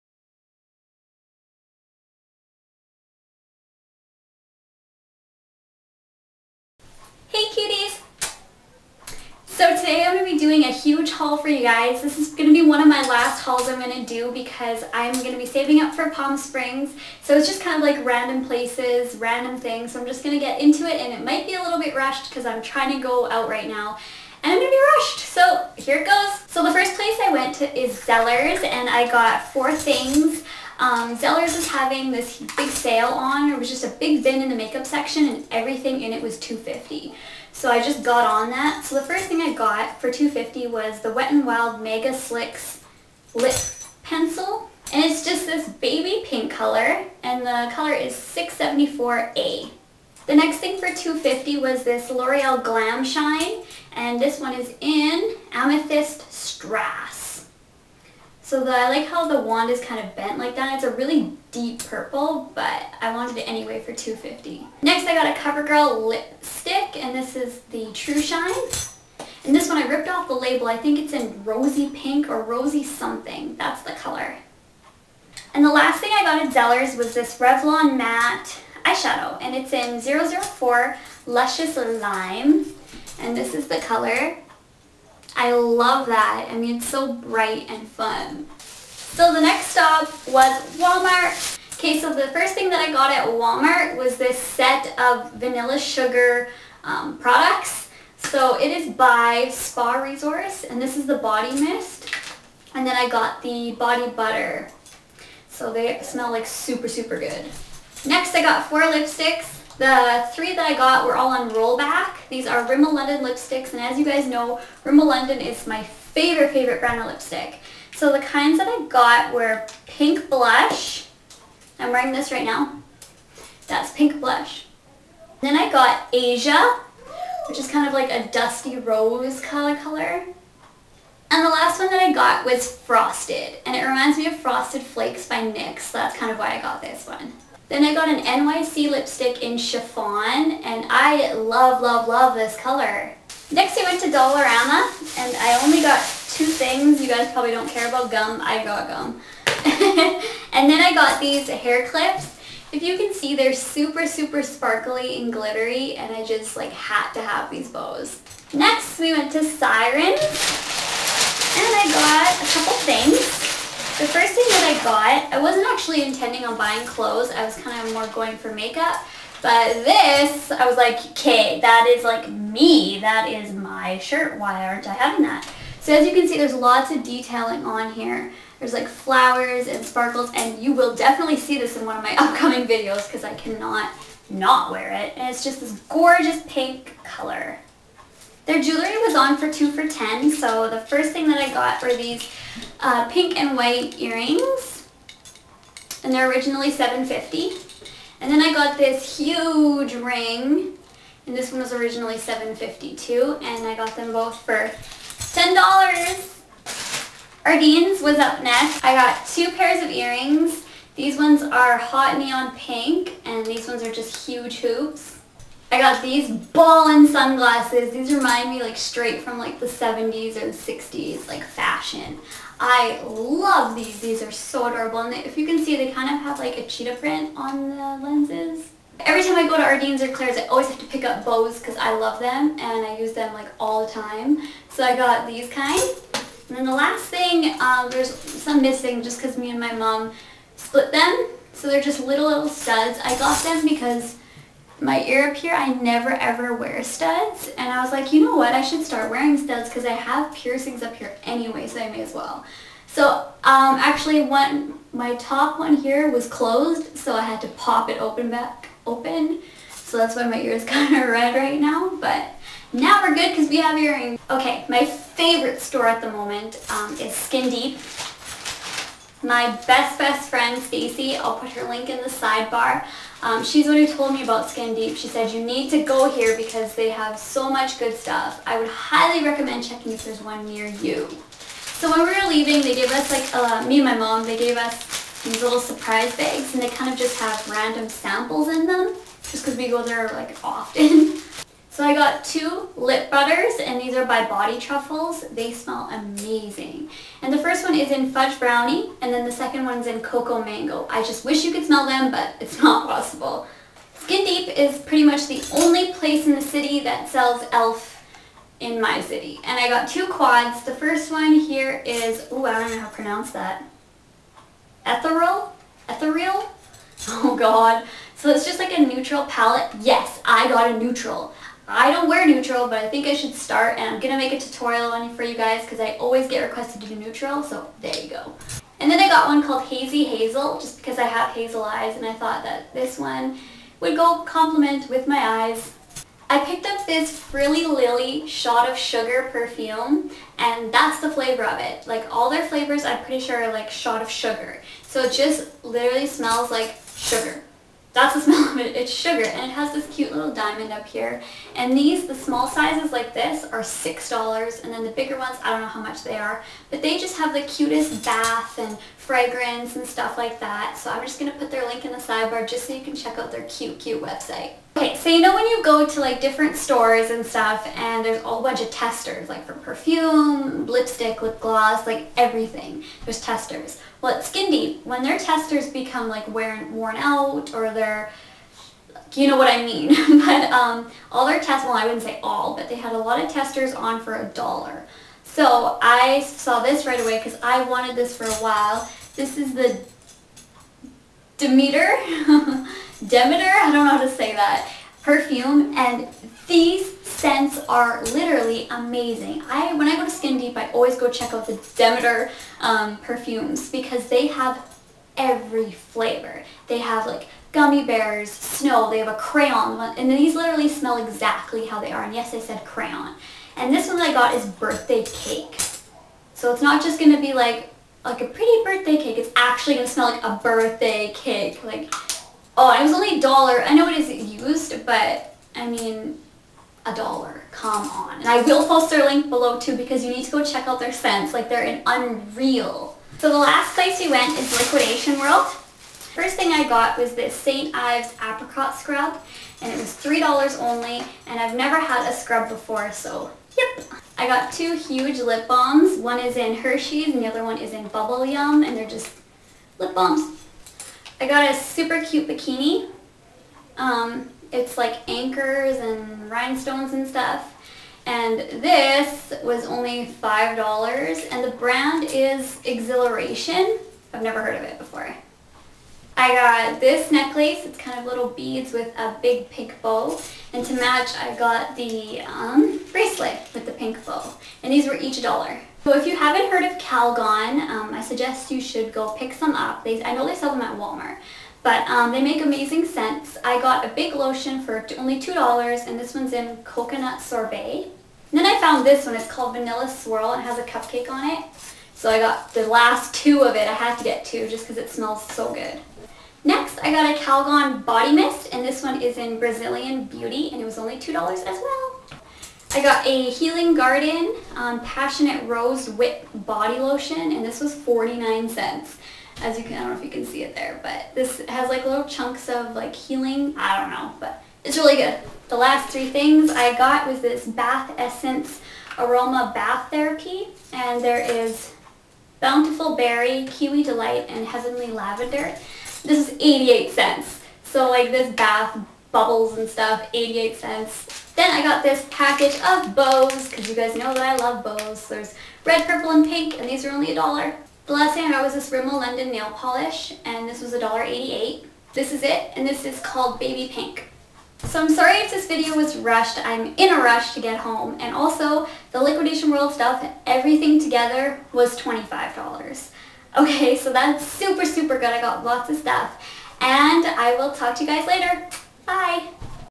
Haul: Zellers, Walmart, Dollarama, Sirens, Ardene, Skin Deep, Liquidation world
-This video is rushed, sorry!-

Zellers:
-Wet n' wild megaslicks lip pencil - 674A
-Loreal glam shine - Amethyst Strass
-Covergirl Trushine lipstick - Rosy shine
-Revlon matte eyeshadow - Luscious lime

Walmart:
-Vanilla sugar body spray
-Vanilla sugar body butter
-Rimmel london lipstick - Pink blush
-Rimmel london lipstick - Asia
-Rimmel london lipstick - Frosted
-NYC lipstick - Chiffon

Dollarama:
-Gum
-Glittery hair bows

Sirens:
-Pink sparkly tank top
-Pink and white flower earrings
-Huge black flower ring

Ardene:
-Neon pink starry earrings
-Huge hoops
-Brown sunglasses
-Blue bows
-Package of studs

Skin deep:
-Body Truffles lip butter - Fudge brownie
-Body Truffles lip butter - Cocoa Mango
-ELF eye quad - Ethereal
-ELF eye quad - Hazy Hazel
-Frilly lilly shot of sugar perfume
-Demeter perfume - Birthday Cake

Liquidation world:
-St. Ives apricot scrub
-Bubble yum lip balm
-Hersheys lip balm
-Blue and pink Anchor bikini - Xhiliration
-Pink bow necklace
-Pink bow bracelet
-Calgon coconut sorbet lotion
-Calgon vanilla swirl lotion
-Calgon body mist Brazilian beauty
-Healing garden passionate rose whipped body lotion
-Bath essence aroma bath therapy
-Cute Hair bows
-Rimmel london nail polish - Baby pink